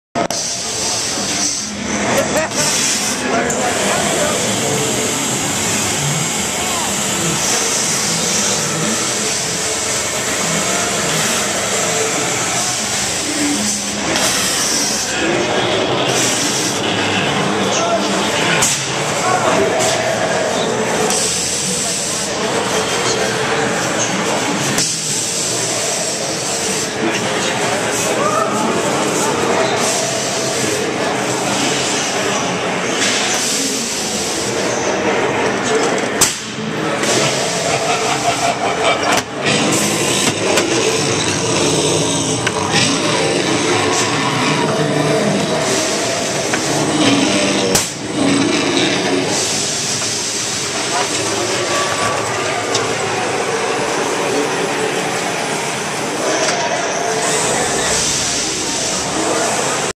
TransWorld Haunters' Convention 2012 "Scarefactory"
Scarefactory does it again with some great animatronics ... follow Lord Grimley's Manor around their booth at TransWorld 2012.

Shop with us on E-Bay and on our website !!